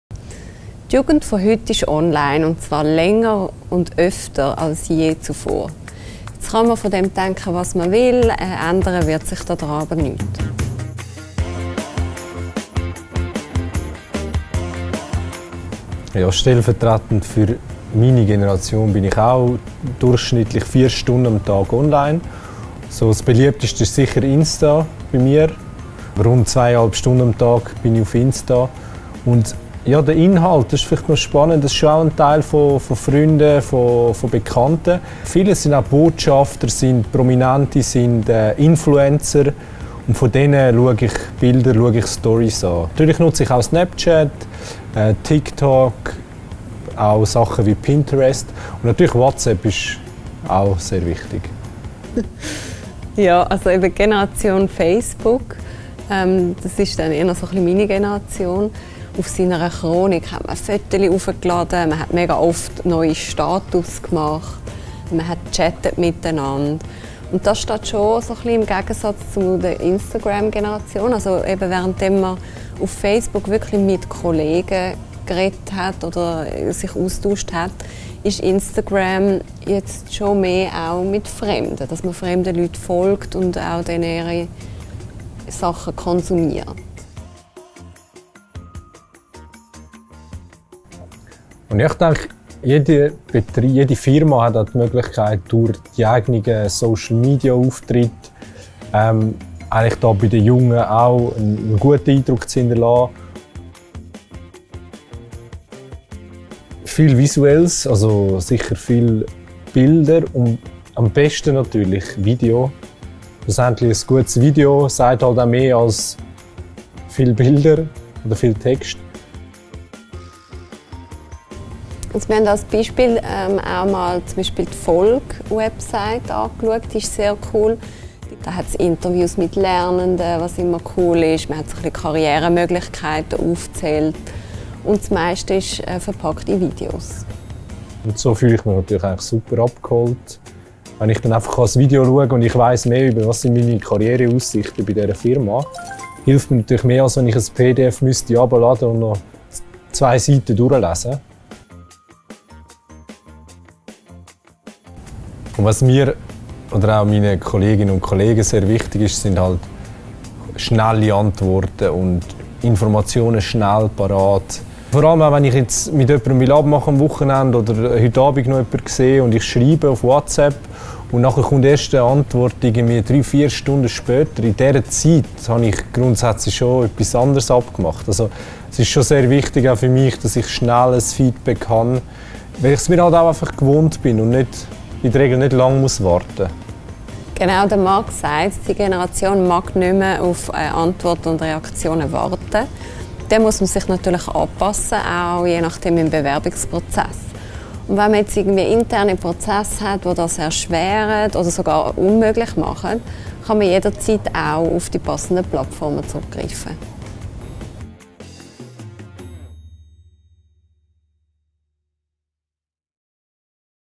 Jugend online - Yousty Talk
Herausforderungen für Lehrbetriebe aufgrund Online-Konsum von Jugendlichen im Jahr 2021:

Corinna und Marc diskutieren heute, wie Jugendliche Inhalte in Bezug auf Lehrstellenmarketing konsumieren und wo man sie erreichen kann.

0:00 Die Jugend ist online!
0:16 Wo & wie lange hält sich die Generation Z online auf
0:42 Social Media-Kanäle für die Generation Z
0:53 Generation Facebook vs. Generation Instagram
1:28 Influencer Marketing
1:44 Online-Auftritt gestalten
2:00 Best practise Website
2:32 Verhalten während Bewerbungsprozess